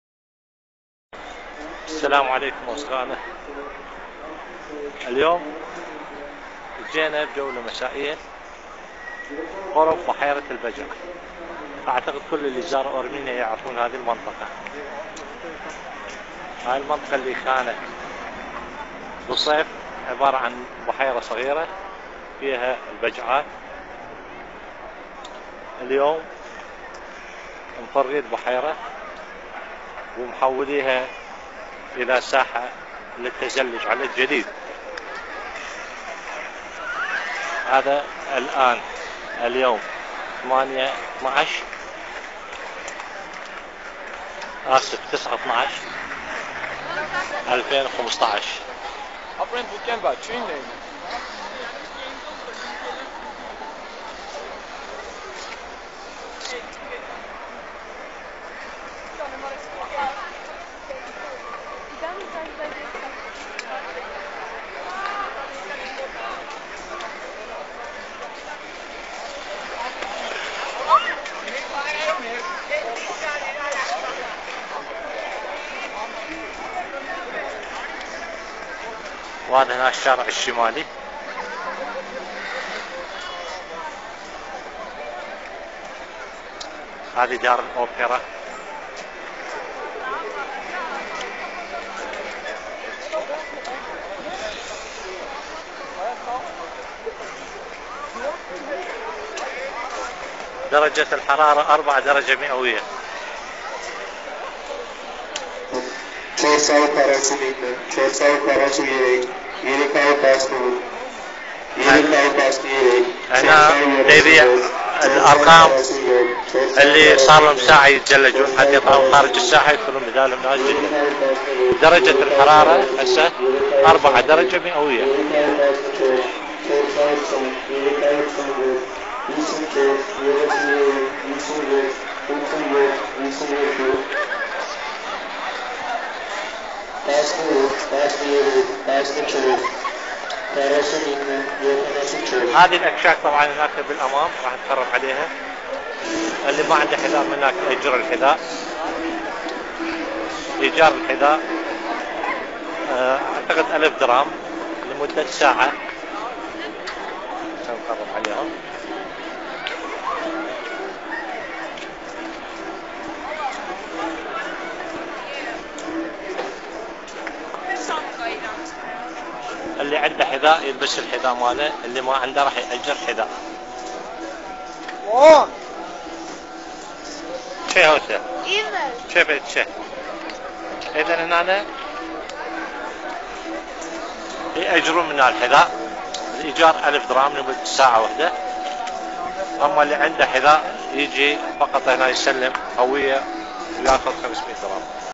ارمينيا / حلبة التزلج على الجليد في يريفان
،بحيرة البجع في وسط يريفان، تتحول قريبا الى حلبة تزلج على الجليد بحيرة البجع لمن لم يعرف موقعها جيدا، تقع في نهاية الجادة الشمالية (  North Avenue) وخلف دار الاوبيرا، وسنويا تسعى بلدية يريفان جاهدا على تهيئة هذه الحلبة لتكون جاهزة في الايام العشرة الاوائل من ديسمبر، وتبقى مفتوحة لغاية 1 مارس،